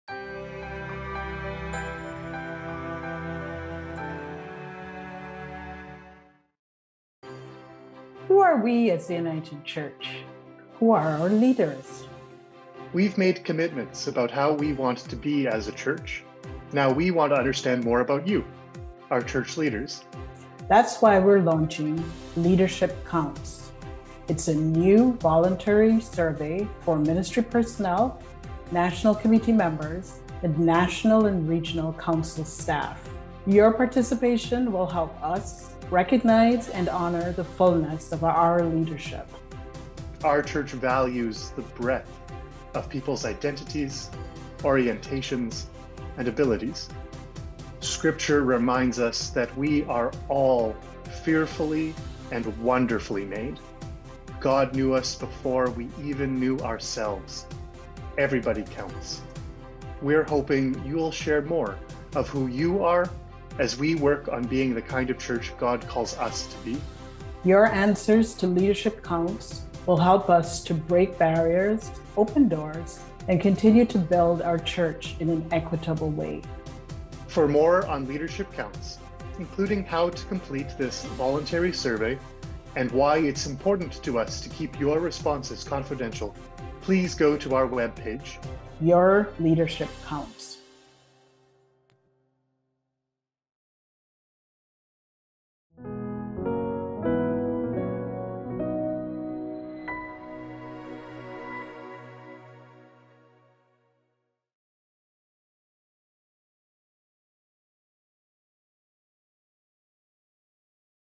Leaders, we'd like to know you better
Leadership Counts is a short confidential, voluntary survey for leaders in churches, national committees, regional and national committees. Help the church celebrate the diversity of its leaders by telling us more about yourself.